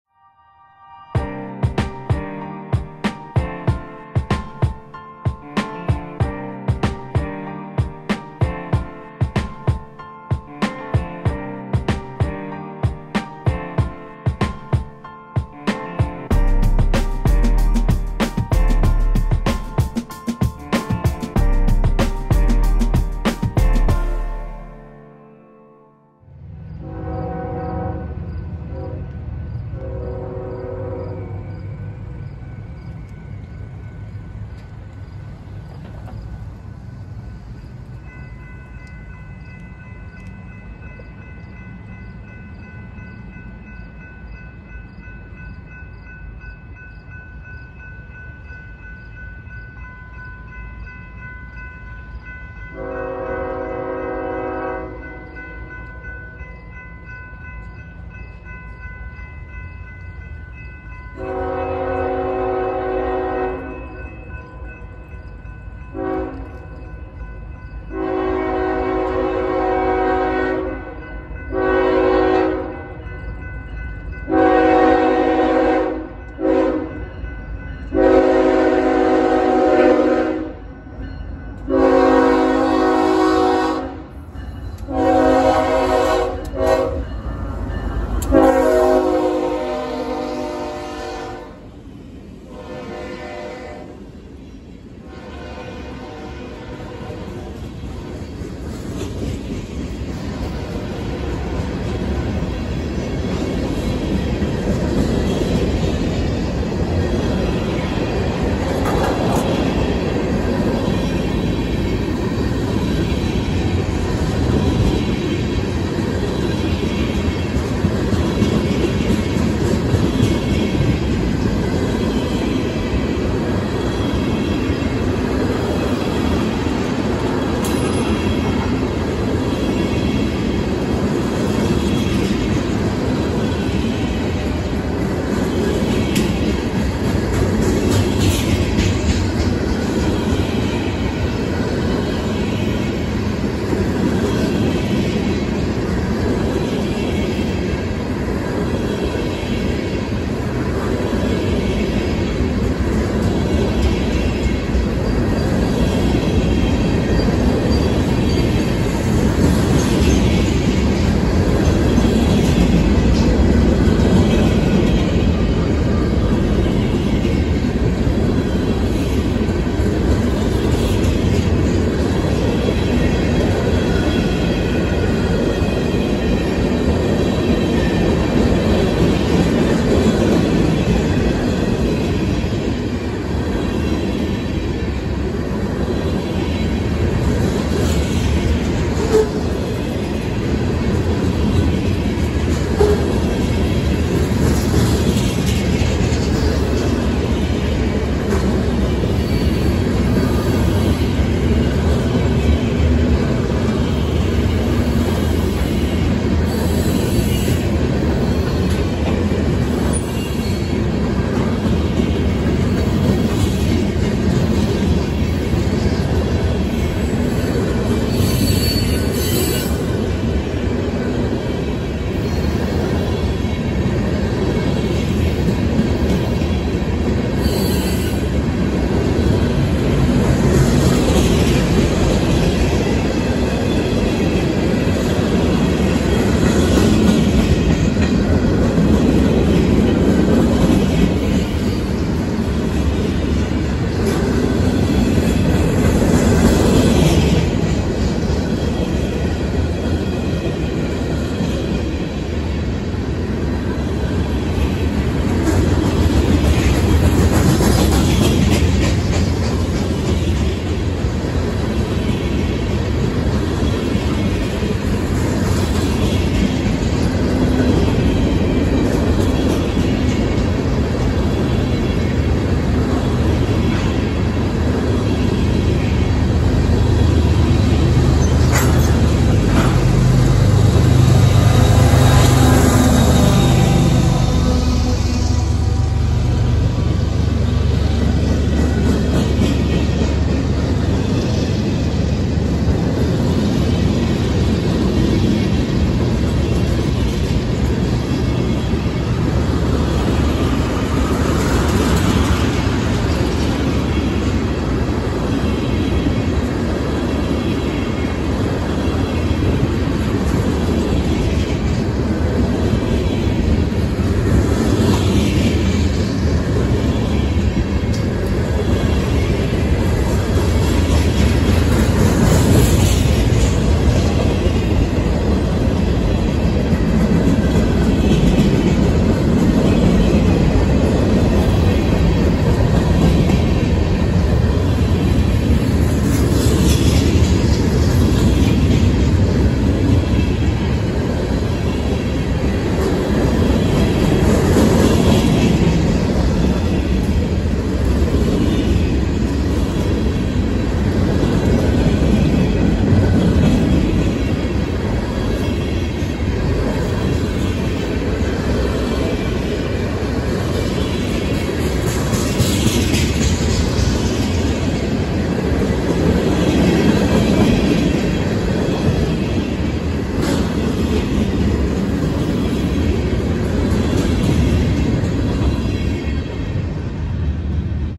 UP 1982 “Missouri Pacific” trails on NS 143 south in Warsaw Indiana 10/5/22
Wooooo Street Running is back and so am I! The thumbnail was taken by my new Nikon camera, hope you enjoy!